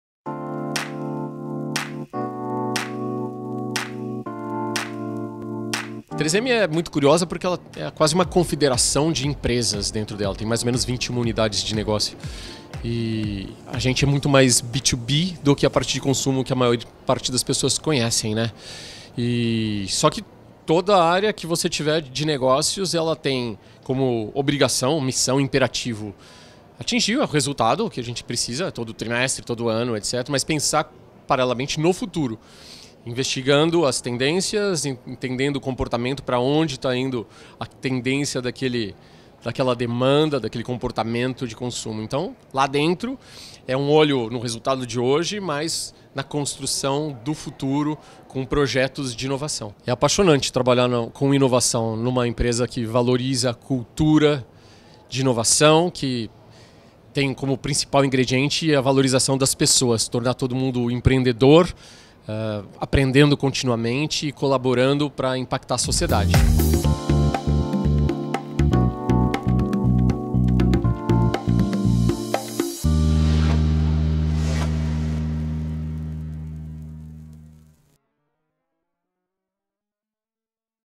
WHOW! Festival entrevista: Luiz Serafim, da 3M do Brasil
O Head de Marca, Digital, Ecommerce e Líder de Inovação de uma das empresas mais inovadoras do mundo fala sobre a importância do tema dentro da companhia!